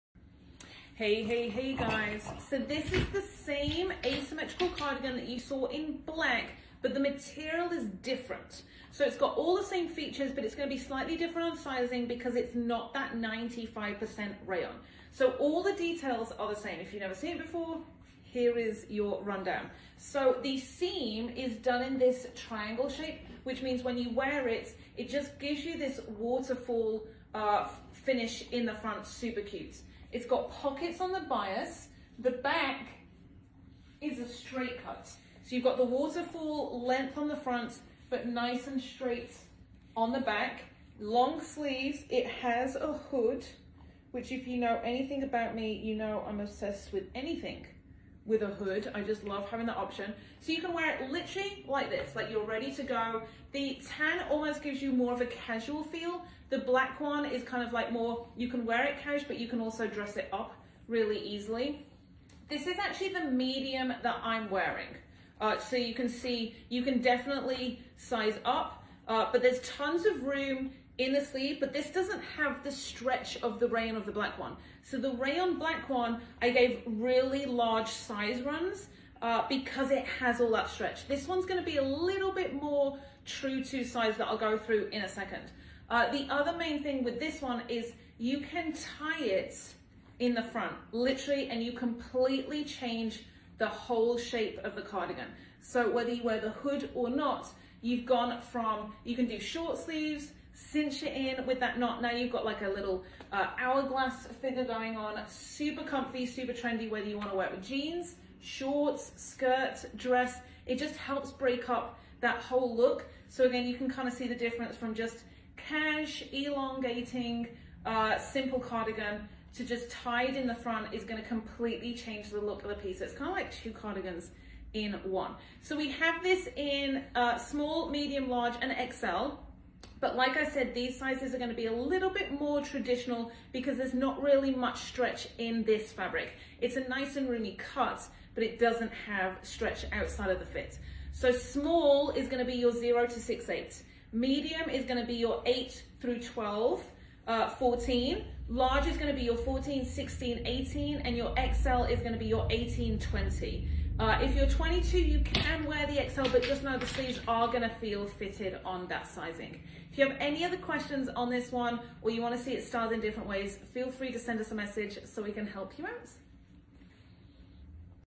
Tan Asymmetric Cardigan Sweater Hoodie with Pockets
A fun, unique waterfall design in this asymmetric cardigan. The addition of the hood and pockets makes it super functional in all conditions as well. Lightweight and a relaxed fit means it's super easy to style, and the thin fabric also makes it super packable. A little thicker than our black version but just as versatile in style and portability -so easy to throw in your messenger bag and have it ready when you need it.

Sizing Recommendations:
Small 0-6/8
Medium 8-14
Large 14-18
XL 18-20

Fabric 54% Rayon 46% Polyester

Download our Free App for the fastest and easiest way to shop and browse! Available on Apple or Android: Apple.britishboutique.net or Android.britishboutique.net